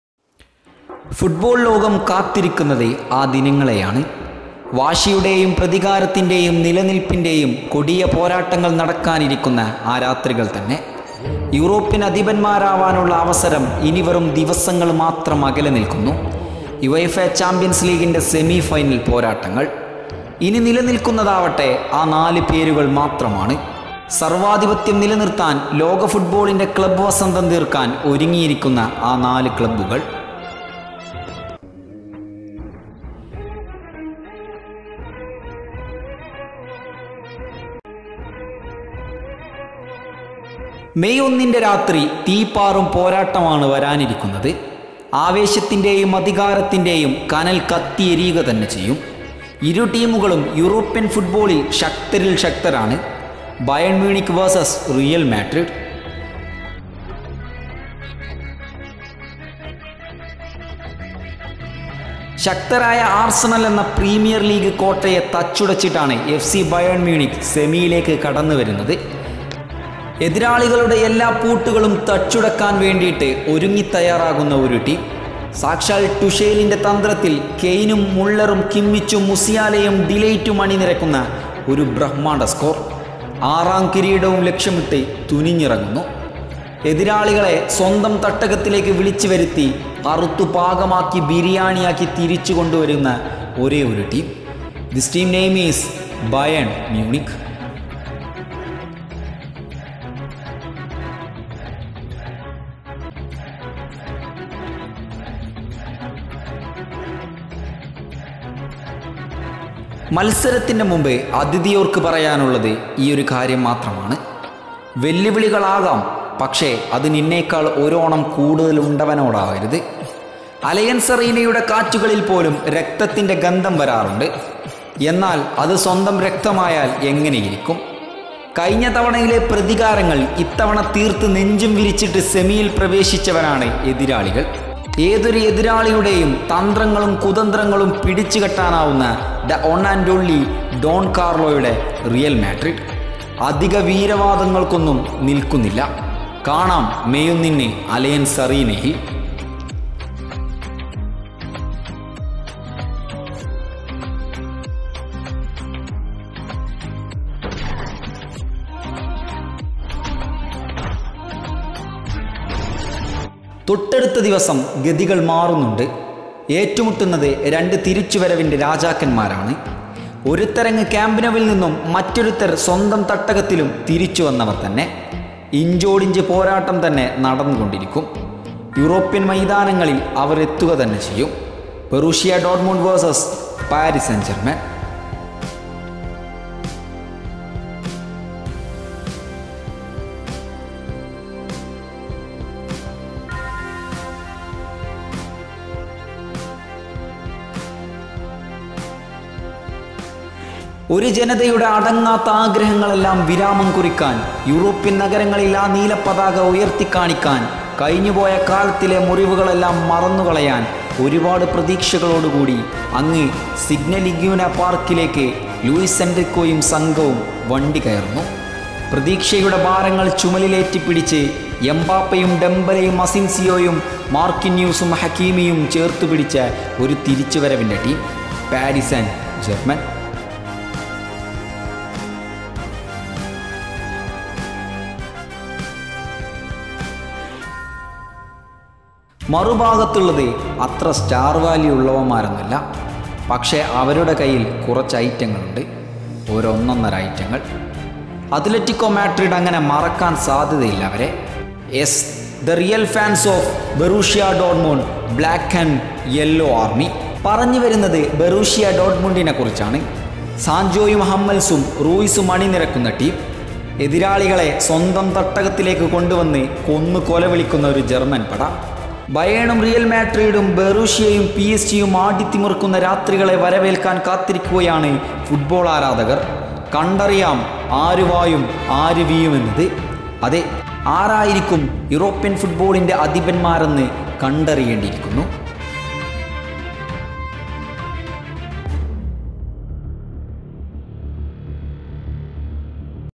"യൂറോപ്പ് ആർക്ക്?. " 🔥UCL Semi Final | Malayalam | Football court...
UEFA Champions League semi final matches live and Malayalam telecasting Malayalam video Bayern Munich versus Real Madrid..